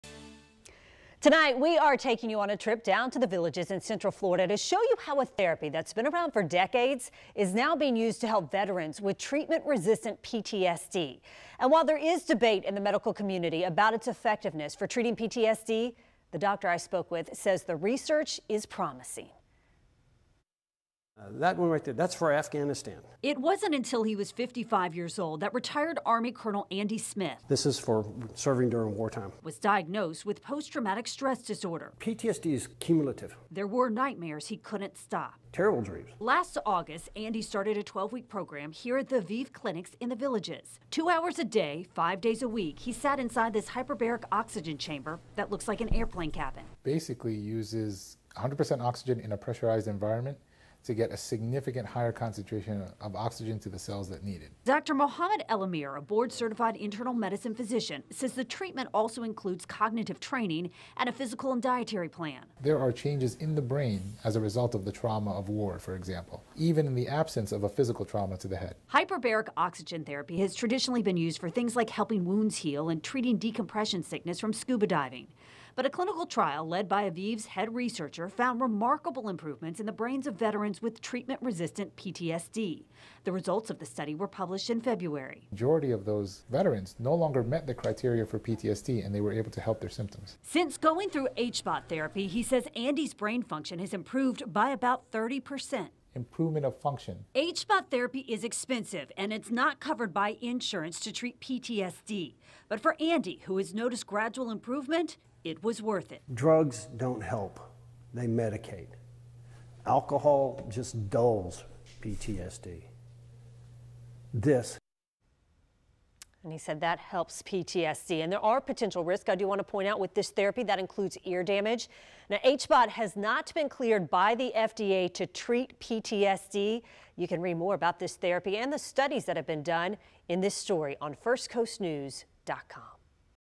Veterans using hyperbaric oxygen therapy to treat PTSD
Doctors and researchers with Aviv Clinics say hyperbaric oxygen therapy is showing promising results for veterans with treatment-resistant PTSD.